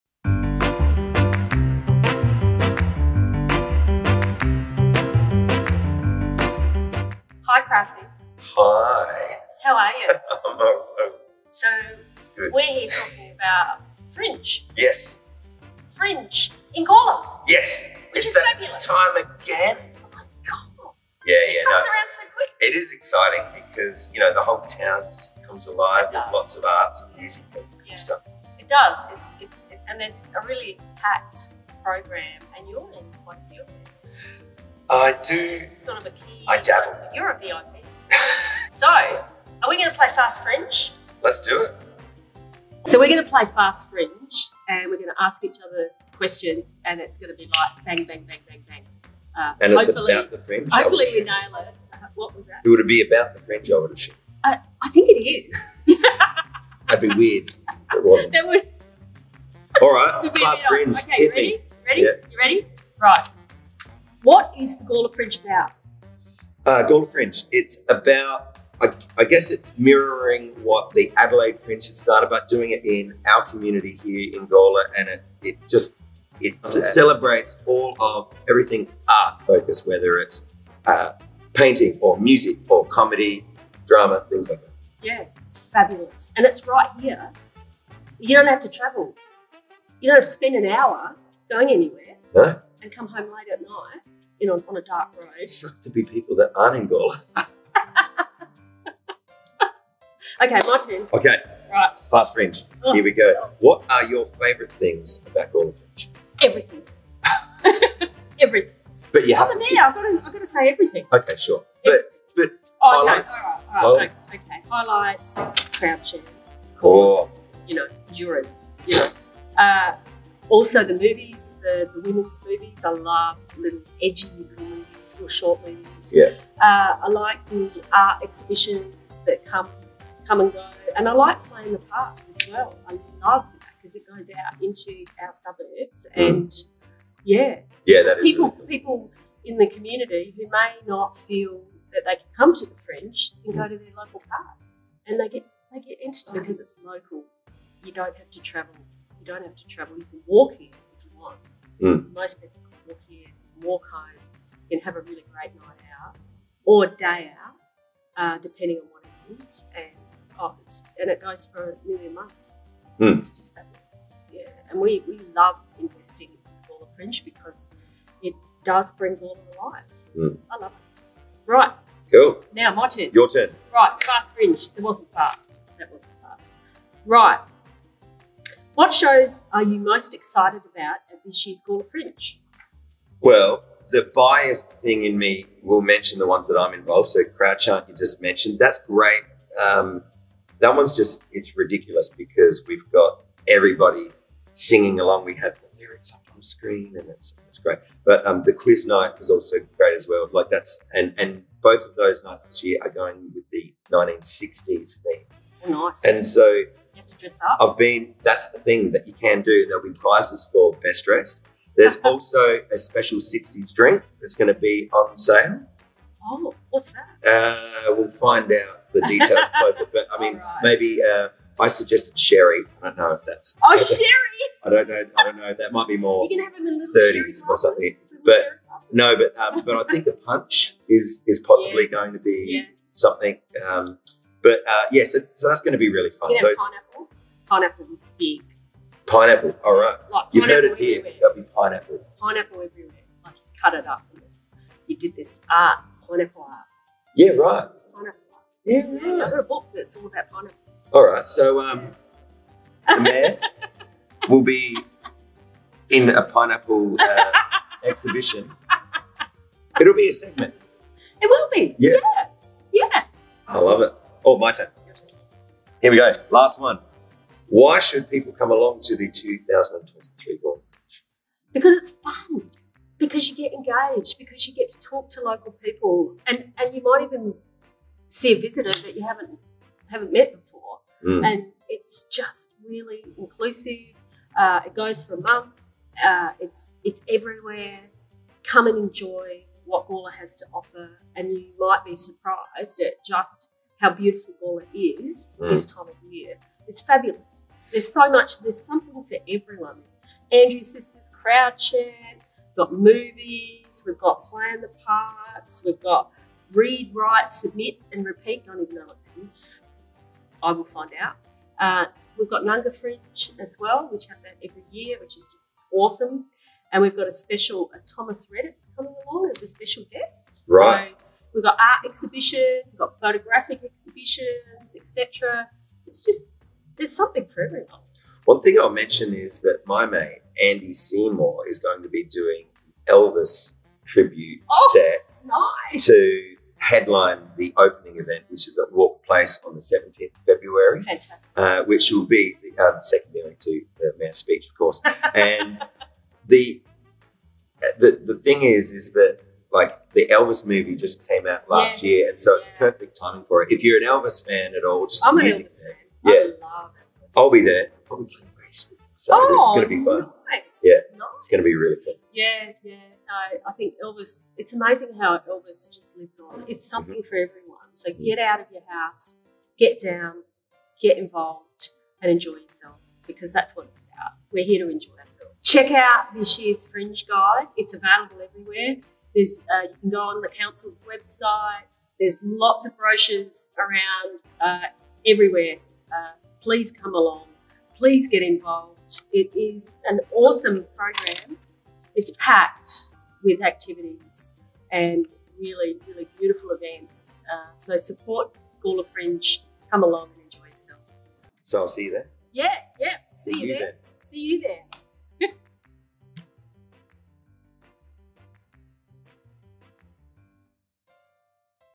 Gawler Fringe 2023 | Mayor Karen Redman and Crafty Play 'Fast Fringe'
Watch as Gawler Mayor Karen Redman and local legend Crafty Music Man sit down to answer some fun questions about the upcoming 2023 Gawler Fringe.

The 2023 program it's jam-packed with a range of fantastic entertainment for the whole family! Tickets for all shows, events and activities taking place both at the Gawler Civic Centre and around the Town of Gawler are now available.

Live music, comedy, film, cabaret, kids and family shows, quiz night, Fringe in the Park, Nunga Fringe performances and activities, the Gawler Fringe Art Exhibition, workshops, demonstrations and more!